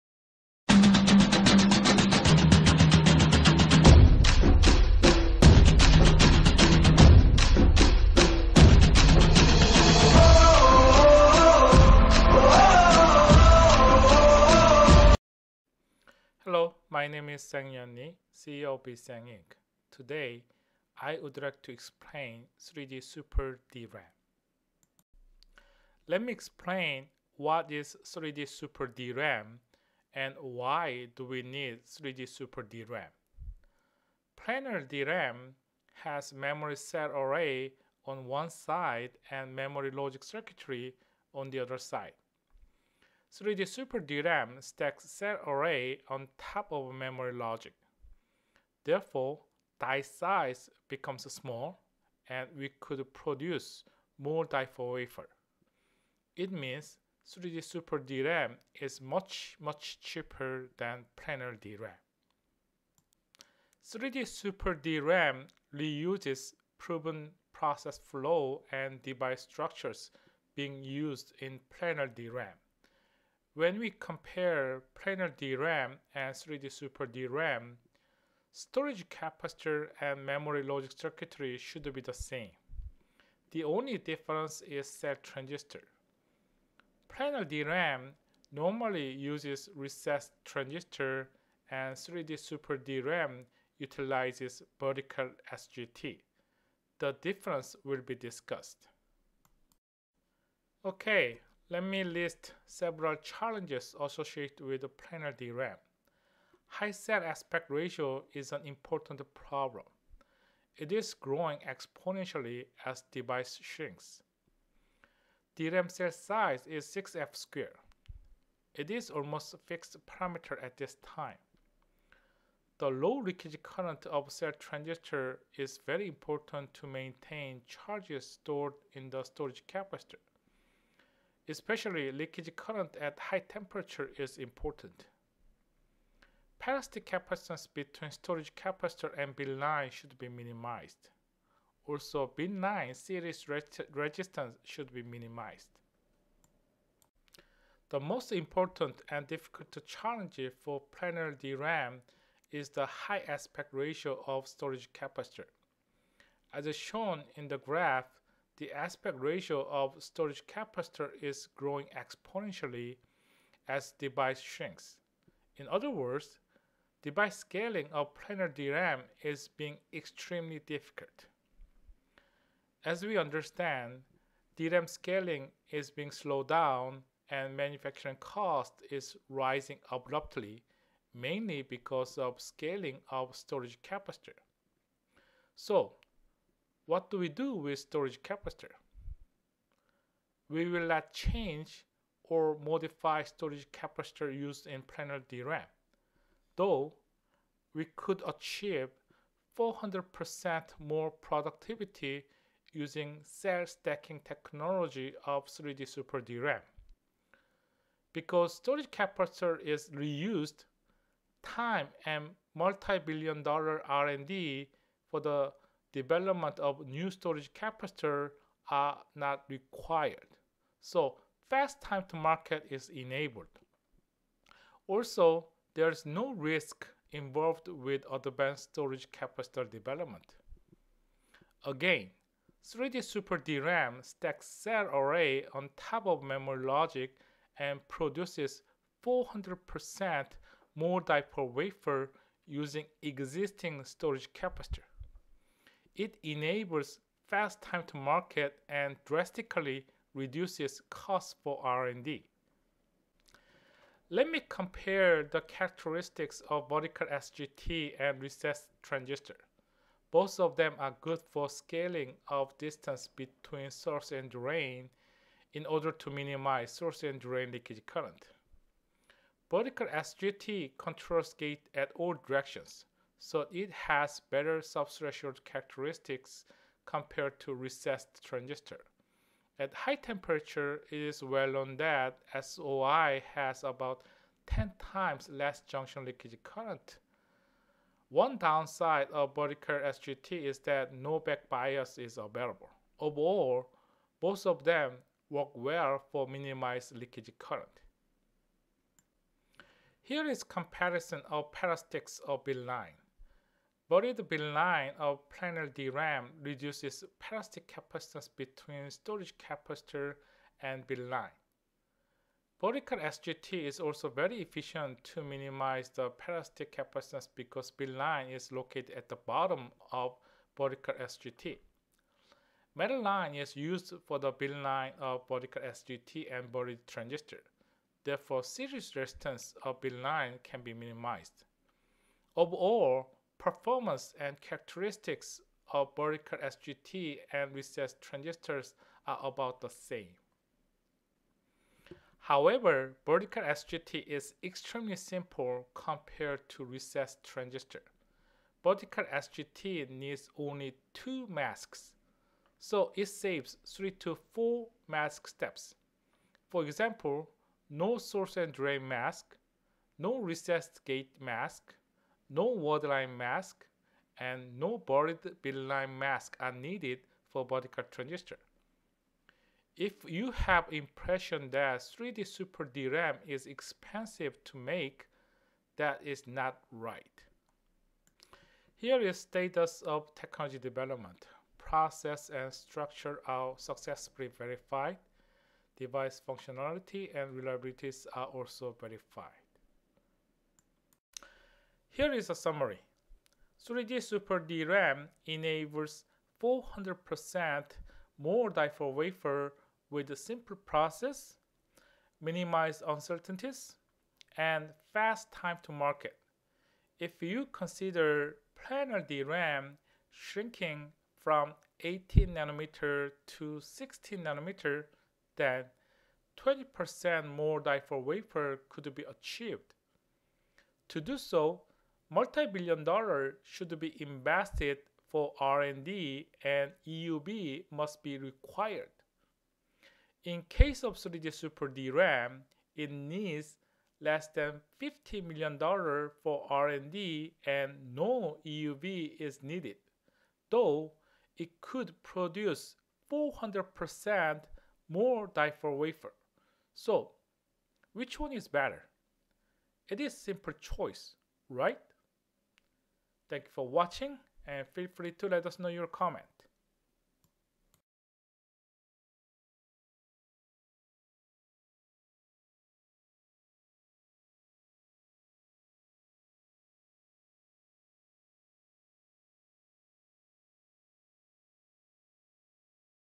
3D Super-DRAM: Ultimate Path to Low Cost Per Bit
Even though 3D NAND does not achieve low cost compared to planar NAND yet, NAND successfully transformed from planar to 3D. However, DRAM still stays in 2-dimension and faces scaling limitation mainly because of scaling of storage capacitor. 3D Super-DRAM could achieve 400% more die-per-wafer using existing storage capacitor and memory logic circuitry. It is simple and enables ultra-low cost.